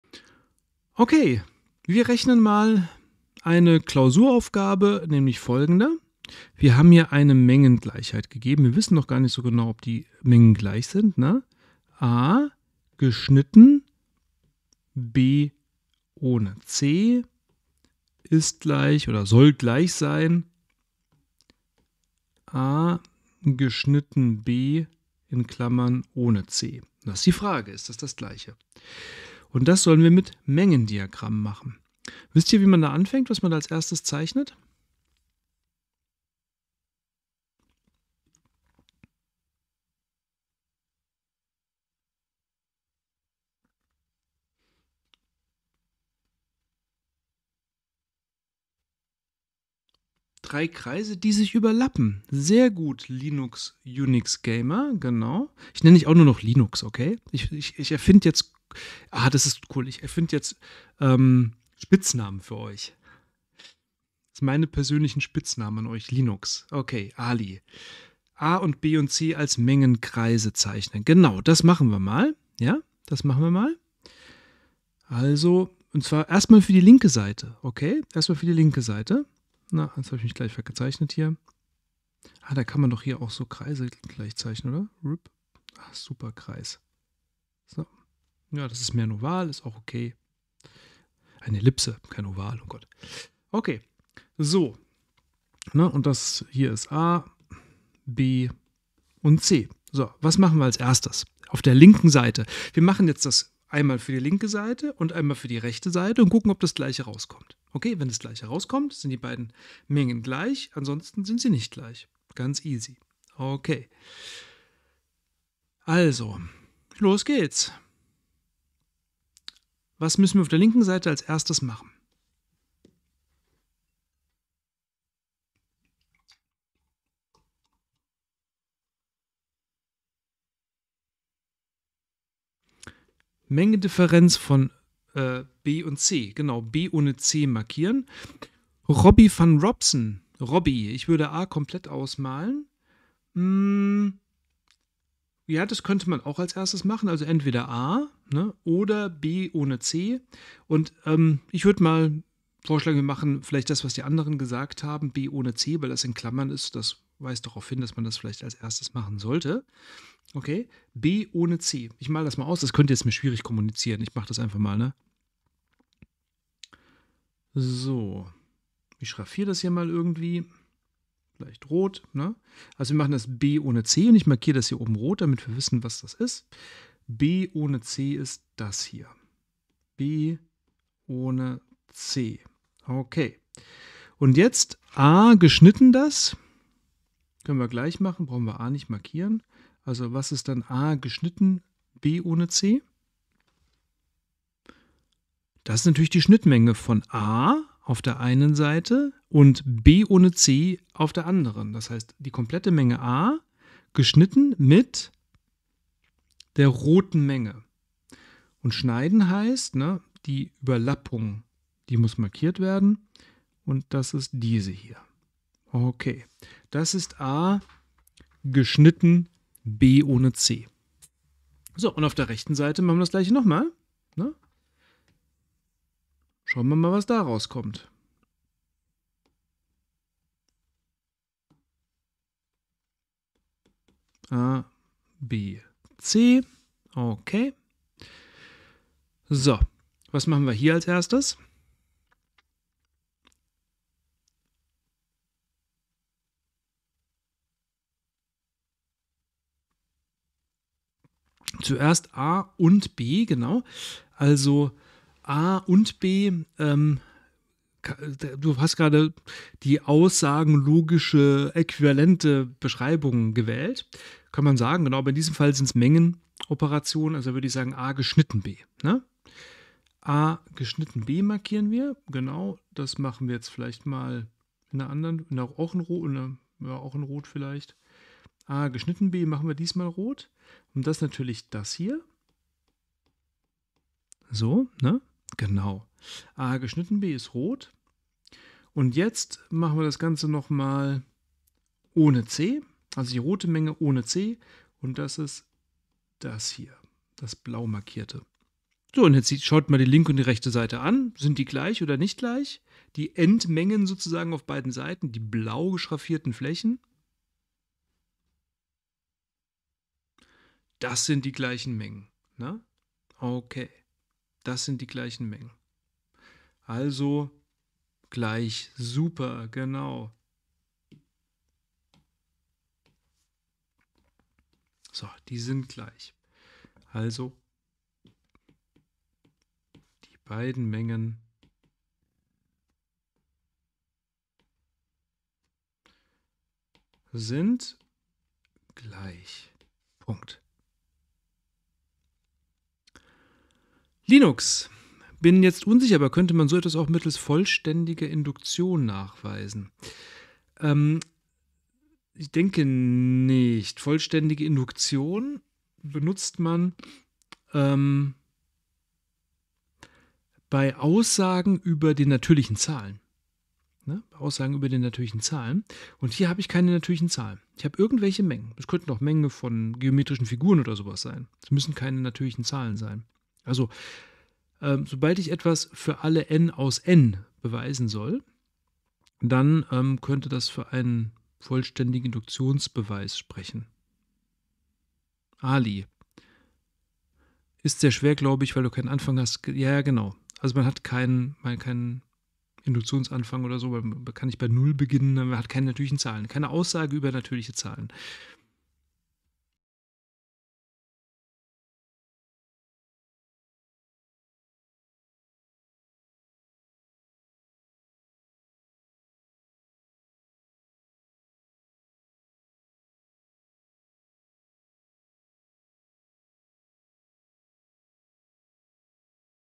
Beweis mit Venn-Diagrammen
🧑‍🏫Heutiges Thema: Wir beweisen eine Mengengleichheit mit Venn-Diagrammen
🎬Regelmäßige Livestreams, auch direkt aus dem Hörsaal!